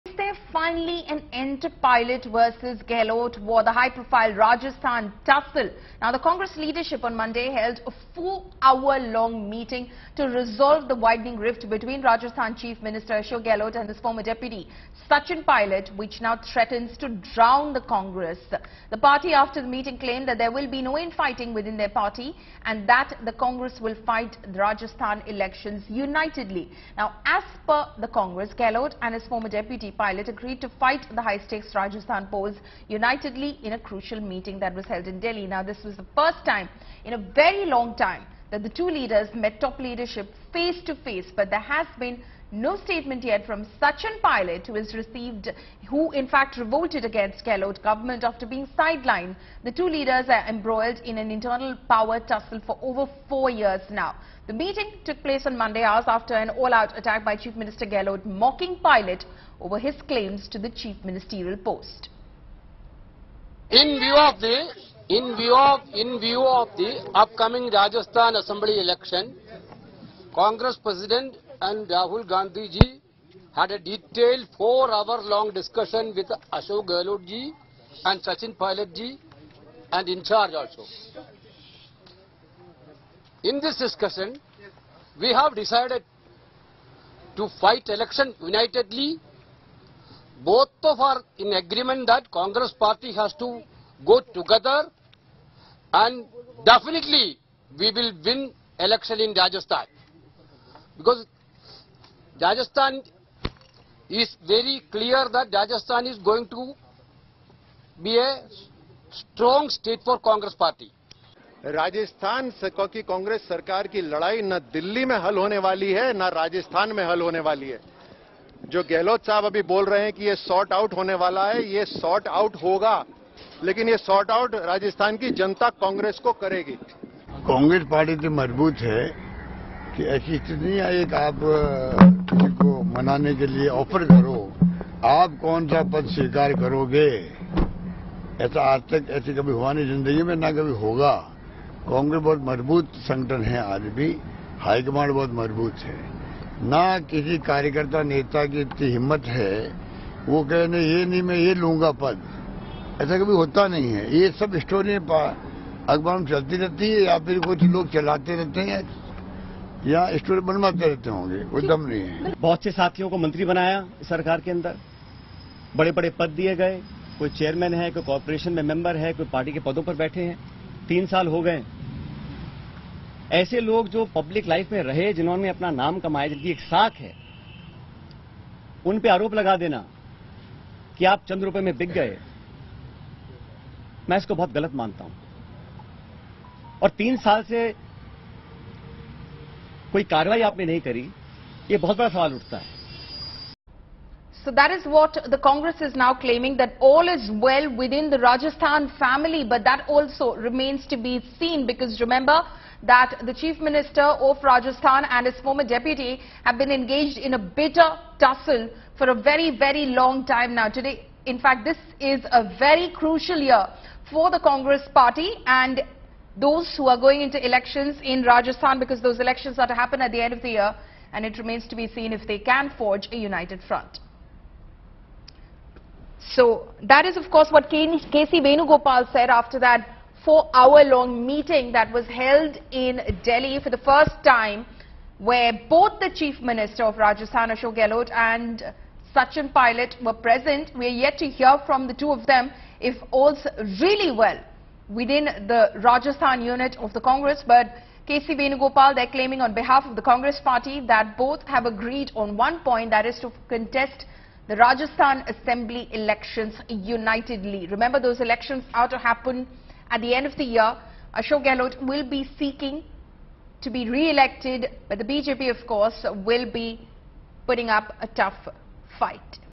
Ashok Gehlot: Congress Doesn't Give Post To Anyone | Temporary Truce In Rajasthan Power Game ?
Rajasthan Chief Minister Ashok Gehlot and his former deputy Sachin Pilot agreed to fight the high-stakes state Assembly polls unitedly in a crucial Congress meeting at party chief Mallikarjun Kharge's residence in New Delhi on Monday.
This was the first time after a long gap that the Gehlot and Pilot met face to face in the presence of the top party leadership. The meeting was called to resolve the infighting in the Rajasthan Congress months ahead of the polls.